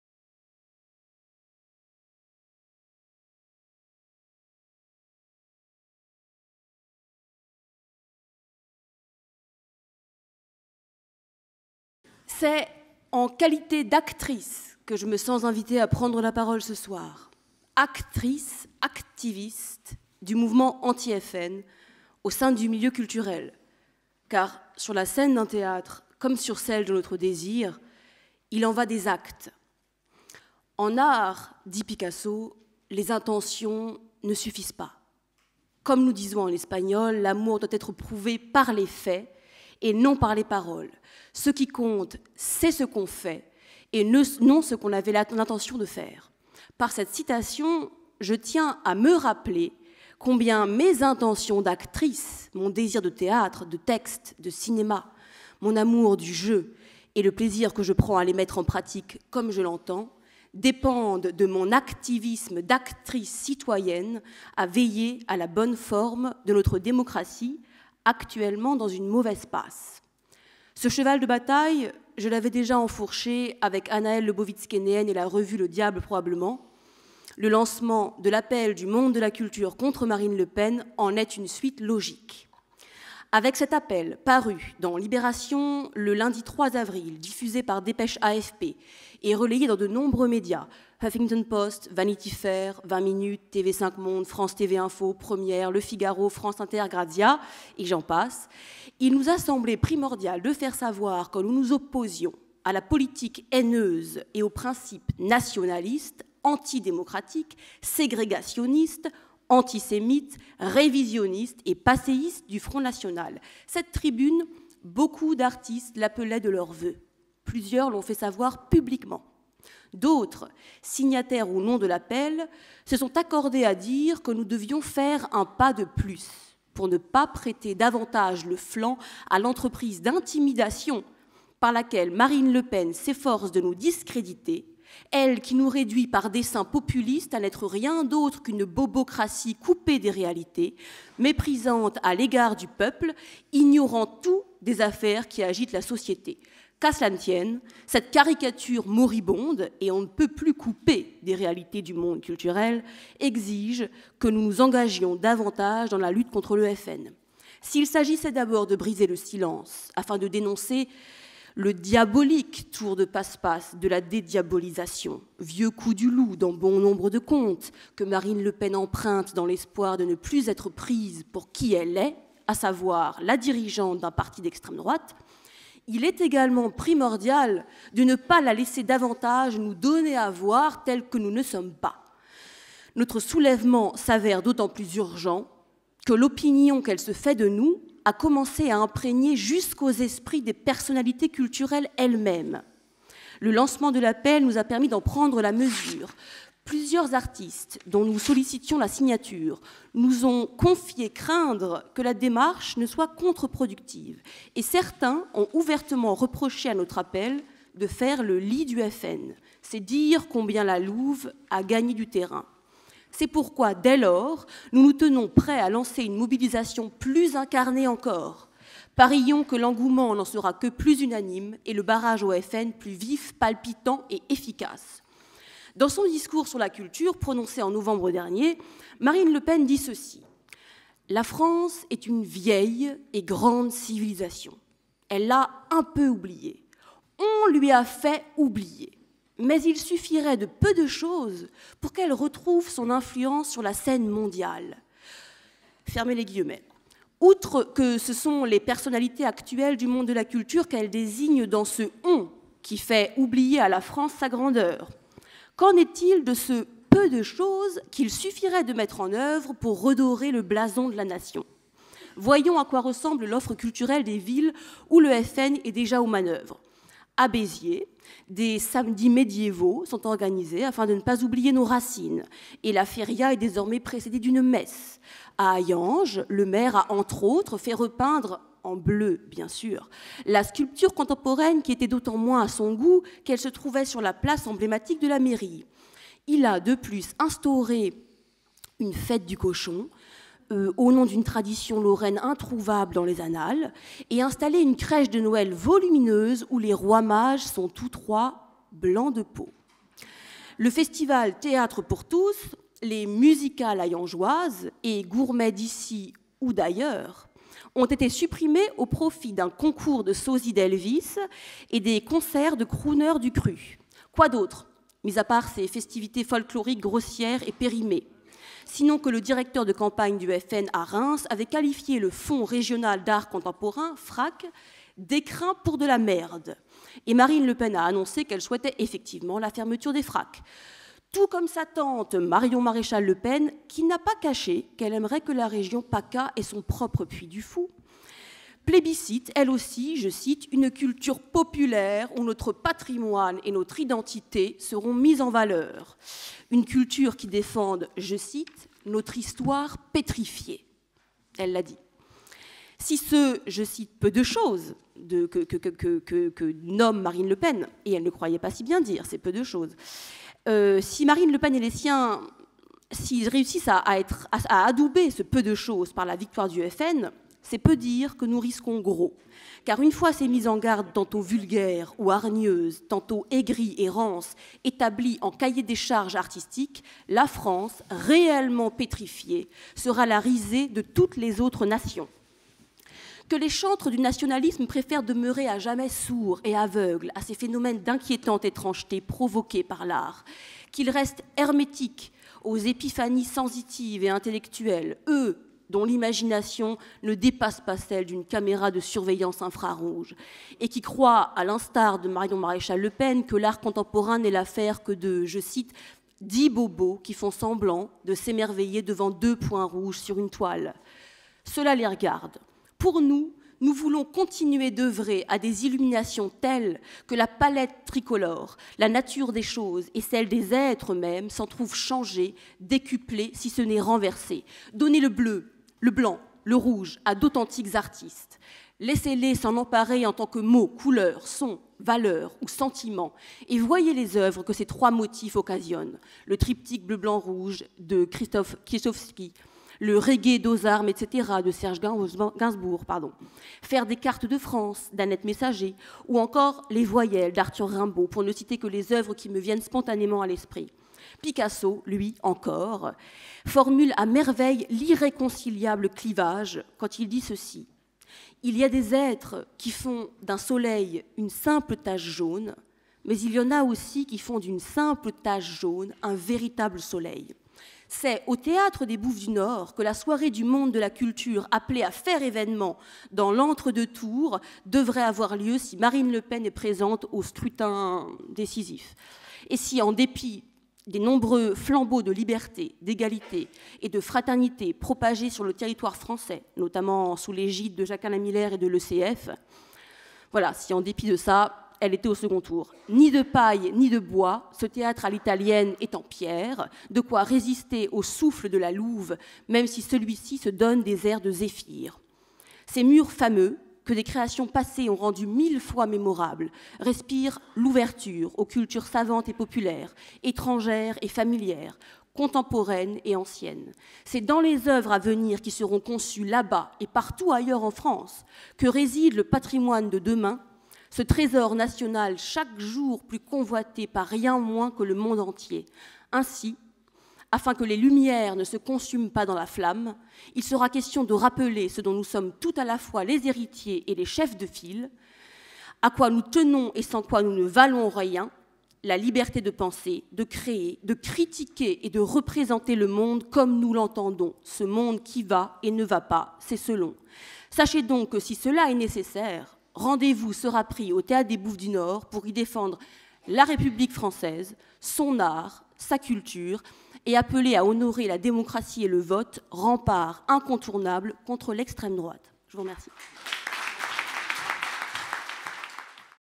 Forum 18 contre Marine Le Pen : Anne Lise Heimburger
L'intervention de Anne Lise Heimburger, lors du Forum contre Marine Le Pen et le parti de la haine, le 18 avril 2017 à la Maison de la Mutualité.
Avec l'Ecole de la cause freudienne et La Règle du jeu.